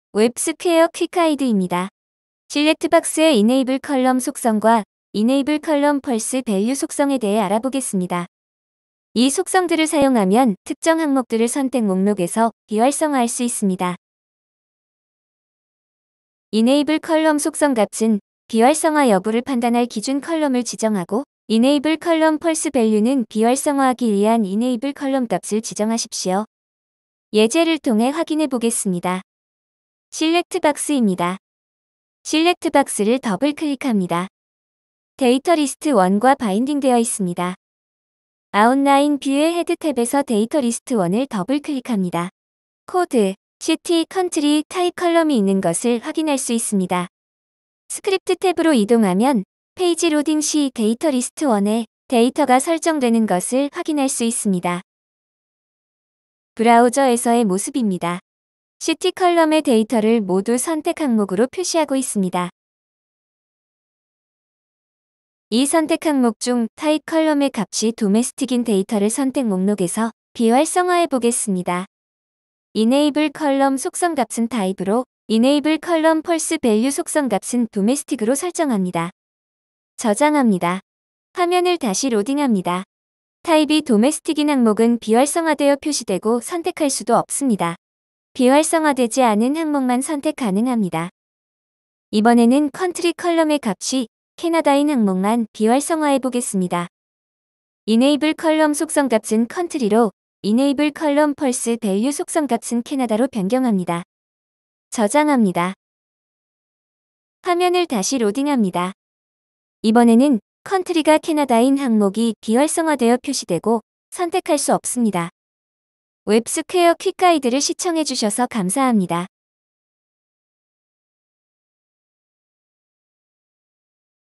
SelectBox - enableColumn & enableColumnFalseValue | WebSquare5 - Quick Guide
00:00 Introduction
00:10 Goal
00:18 Related Properties
00:27 DEMO
01:55 End of DEMO

Disable certain selection items.

Related APIs
   enableColumn
   enableColumnFalseValue